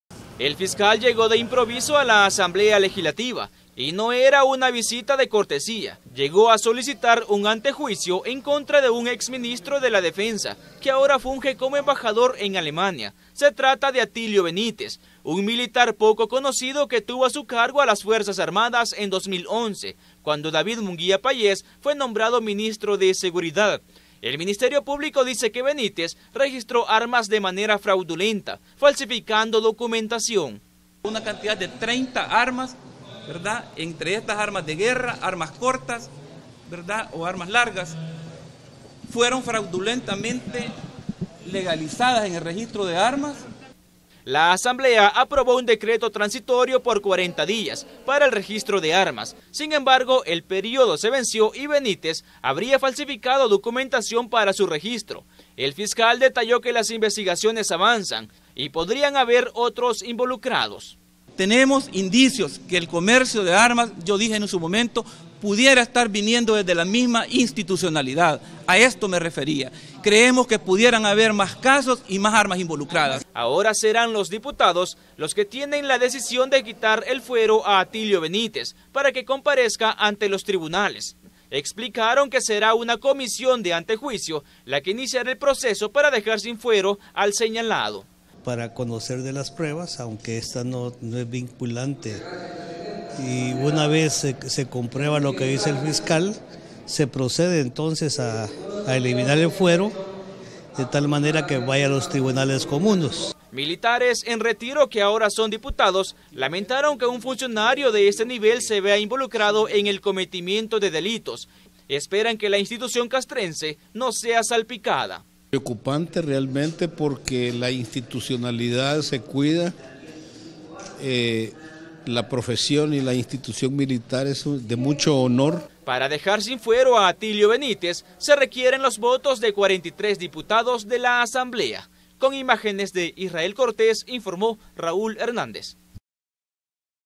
CN. Fiscalía pide antejuicio contra José Benítez
Sopresivamente el Fiscal General de la República solicitó el proceso de antejuicio en contra del actual diplomático José Atilio Benítez, ex ministro de la defensa nacional, por tráfico ilícito de armas.